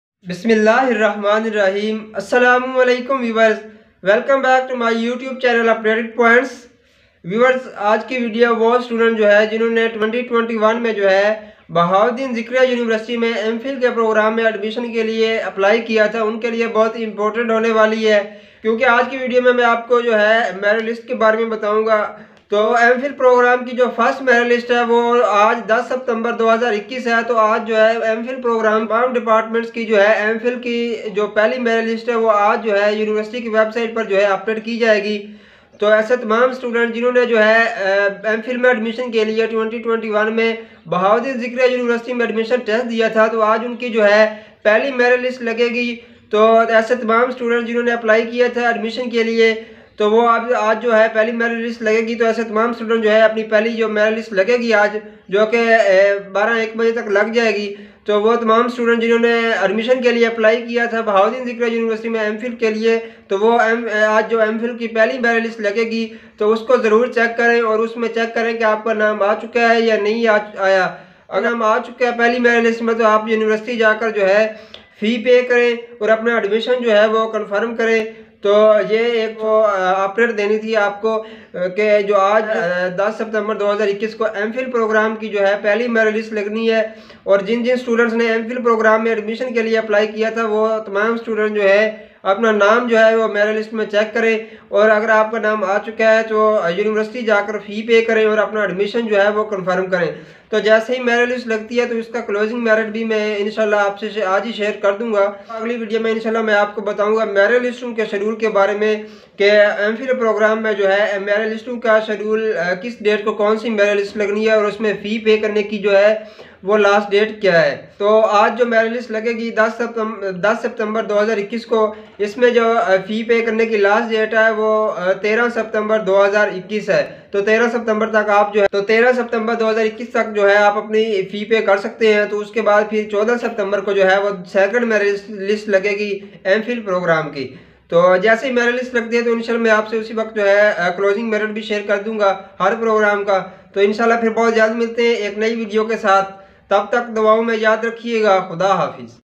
BZU Mphil 1st Merit list Update 2021||Updated Points
BZU Mphil 1st Merit list Update 2021||Merit list mphil BZU 2021

In this video I have talk about BZU Mphil First Merit List 2021.